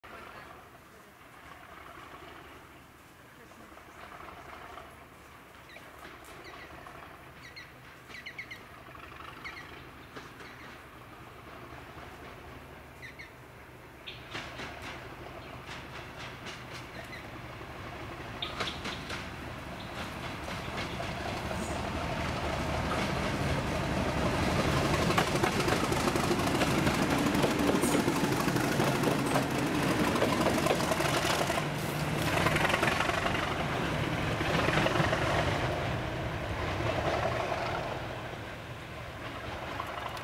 37425 'Concrete Bob' passes Moira West Jn | 15/07/21
37425 'Concrete Bob' passes Moira West Junction working OZ55 from Stowmarket - Stowmarket.